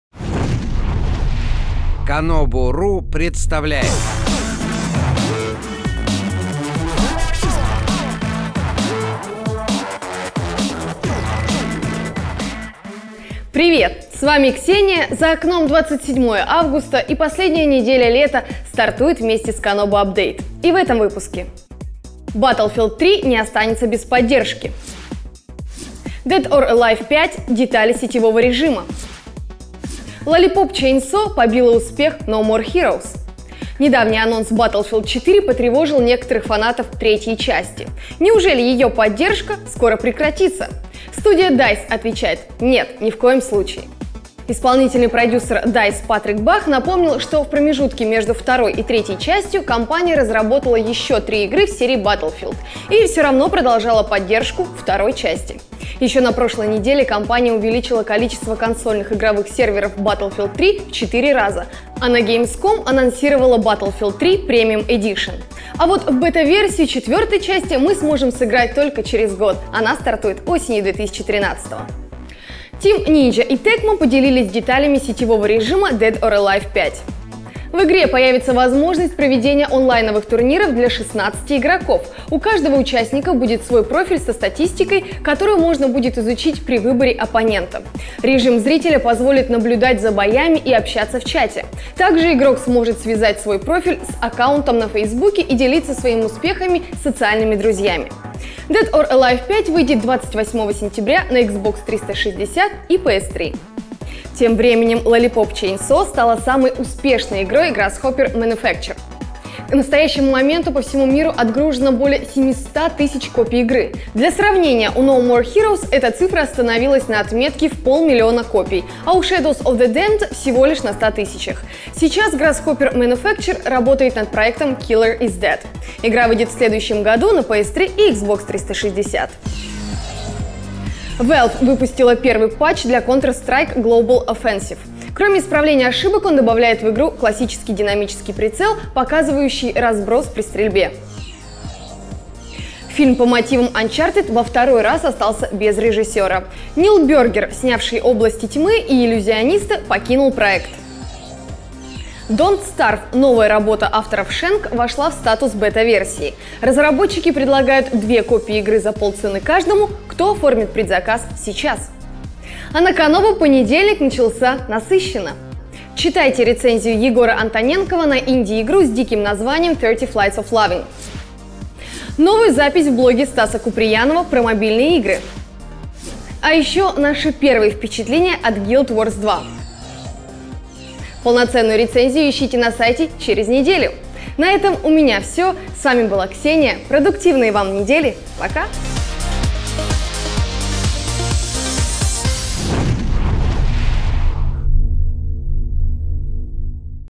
Kanobu.Update (27.08.12)
Присоединяйтесь к обсуждению:
В новом Kanobu.Update: EA не планируют забрасывать Battlefield 3, детали сетевого режима Dead or Alive 5, Lollipop Chainsaw ставит рекорды.